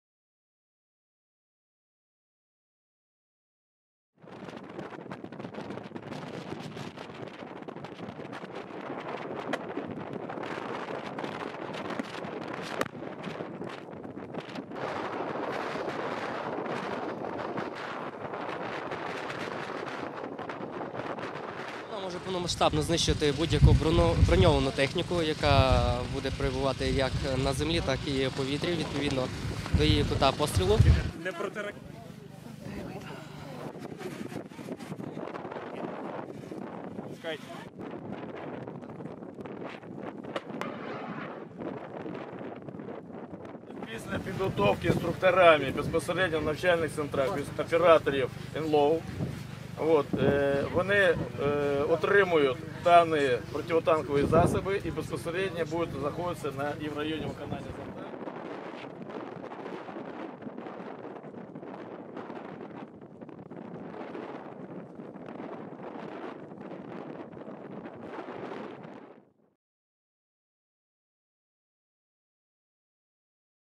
Ukrainian Soldiers Testing Anti-Tank weapons
Ukrainian soldiers testing Anti-Tank weapons supplied by the United States and the UK.